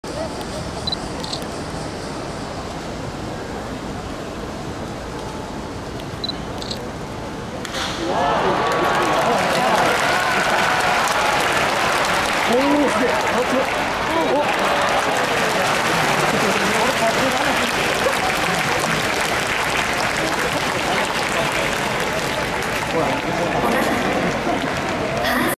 2021/7/29 エチェバリア選手 レフト前タイムリーヒット 第1打席 甲子園球場 阪神タイガース VS 千葉ロッテマリーンズ
打球の行方を見失ってしまいました。
レフト前でなくセンター前かもしれません。